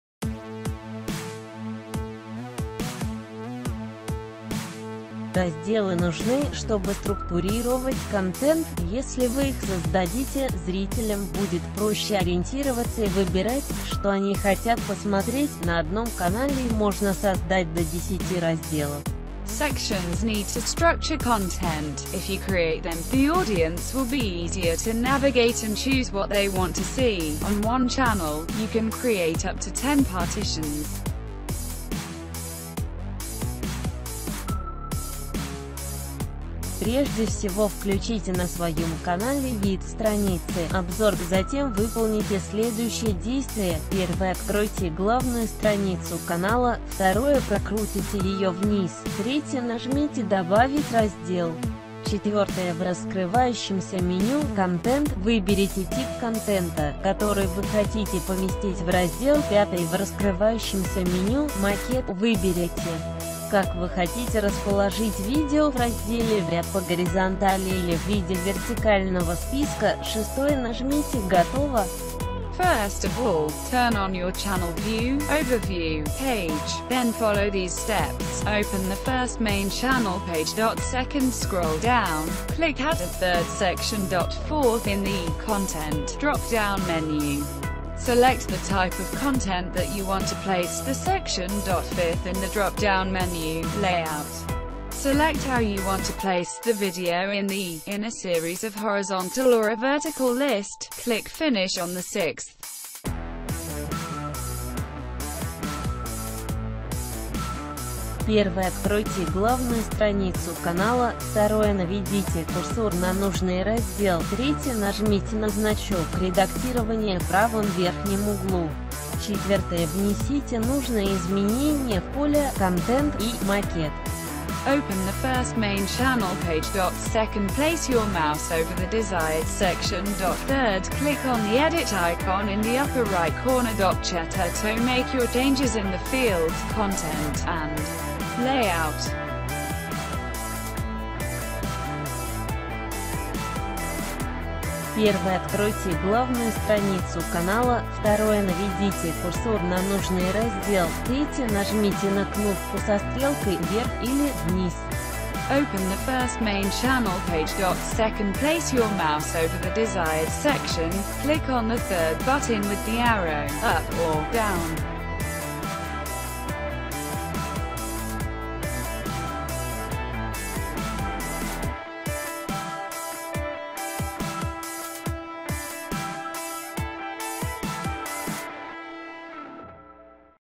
Разделы канала
Разделы нужны, чтобы структурировать контент. Если вы их создадите, зрителям будет проще ориентироваться и выбирать, что они хотят посмотреть. На одном канале можно создать до 10 разделов.

Управление разделами
00:36 - Как создать раздел
Прежде всего включите на своем канале вид страницы "Обзор". Затем выполните следующие действия:

1 - Откройте главную страницу канала.
2 - Прокрутите ее вниз.
3 - Нажмите Добавить раздел.
4 - В раскрывающемся меню "Контент" выберите тип контента, который вы хотите поместить в раздел.
5 - В раскрывающемся меню "Макет" выберите, как вы хотите расположить видео в разделе – в ряд по горизонтали или в виде вертикального списка.
6 - Нажмите Готово.

01:47 - Как изменить раздел
1 - Откройте главную страницу канала.
2 - Наведите курсор на нужный раздел.
3 - Нажмите на значок редактирования  в правом верхнем углу.
4 - Внесите нужные изменения в поля "Контент" и "Макет".

02:24 - Как изменить порядок разделов
1 - Откройте главную страницу канала.
2 - Наведите курсор на нужный раздел.
3 - Нажмите на кнопку со стрелкой "вверх" или "вниз".